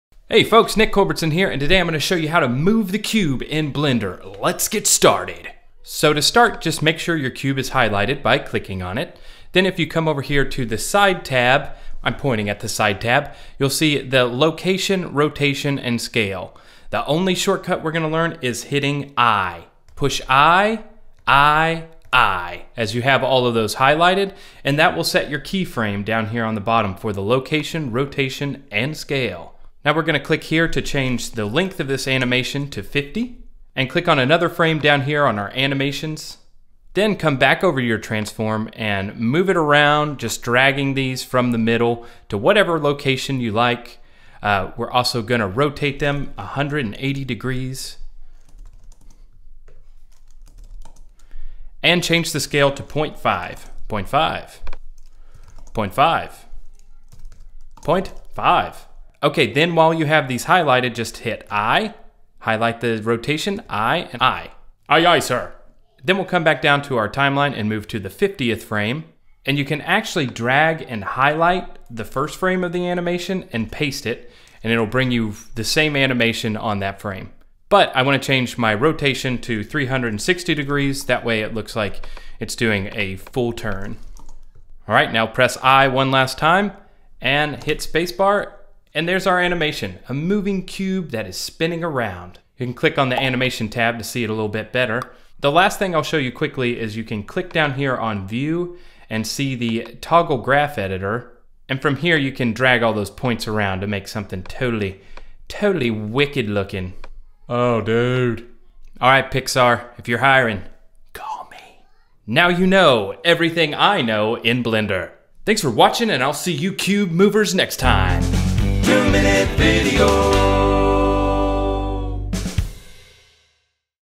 How to Move the Cube in Blender | Simple Animation Tutorial
In this video, I show how to easily animate the cube in Blender. Follow these simple steps for setting keyframes, and you'll be flippin' and a' flappin' that cube all over the dad-gum place!

I picked Blender back up recently and couldn't remember how to apply keyframes to an object. In making/watching this video, the knowledge is now seared into our brains. We will not forget, again. Remember the CUBE!

Hey, Listen! If you like this video, check out the other videos on my channel!

Nick
Moby Pixel